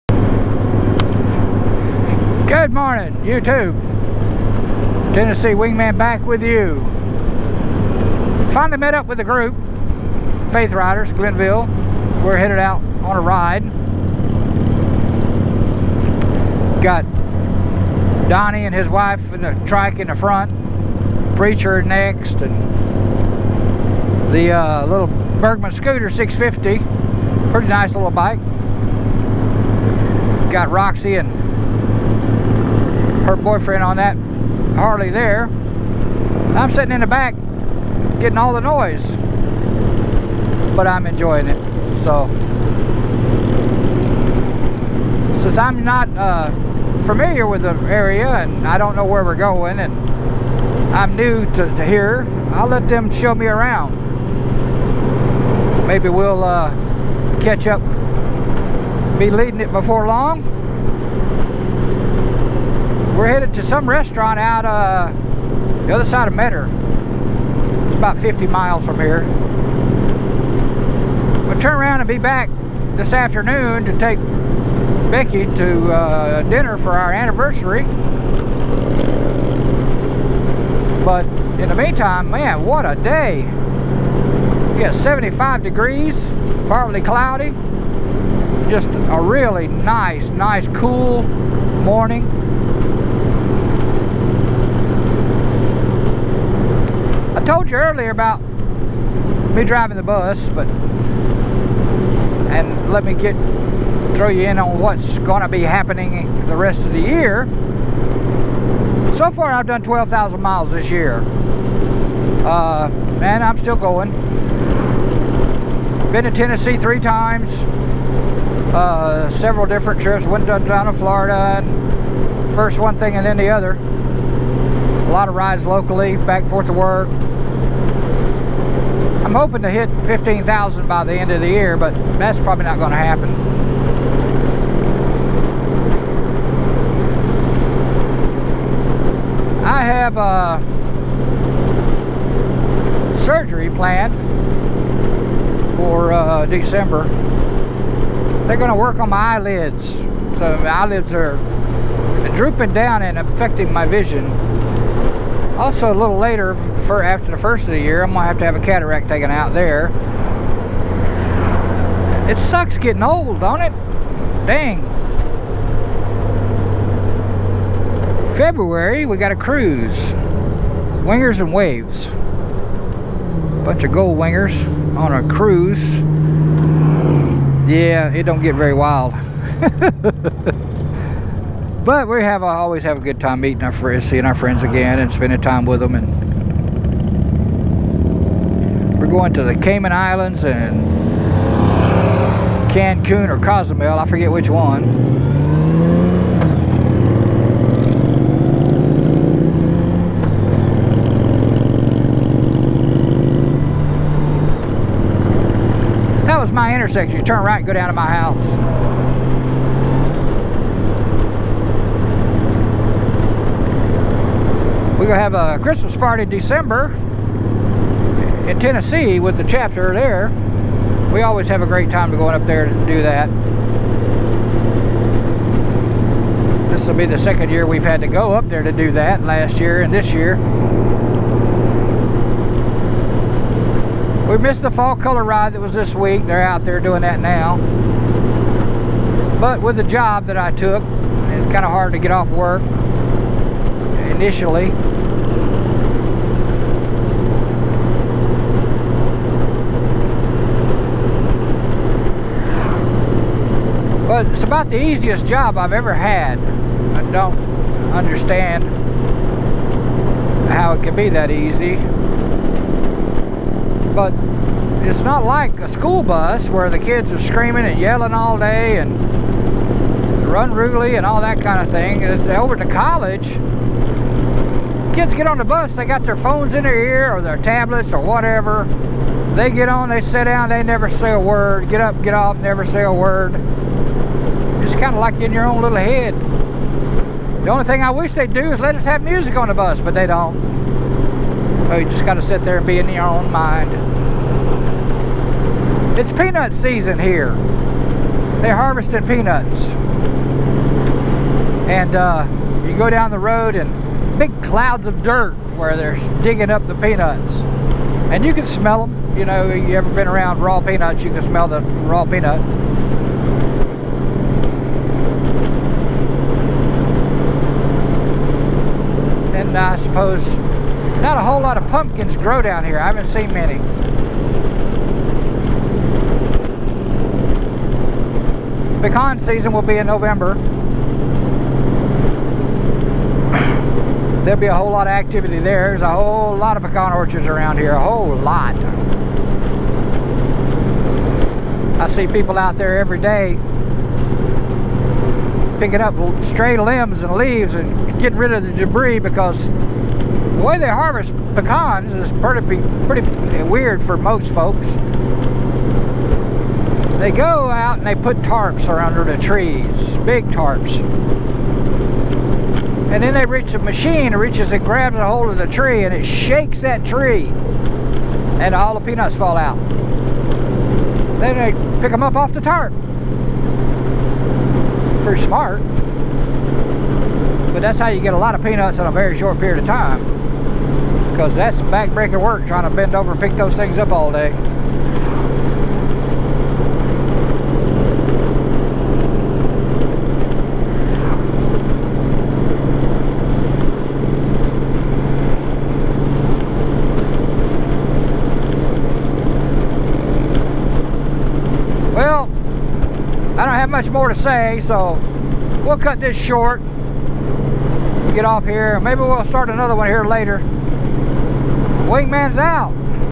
Peanuts and Pecans, OH My
Rambling on a group ride,  Just so you know when I said they shake the peanuts out of the trees, I really meant to say, Pecans.  LOL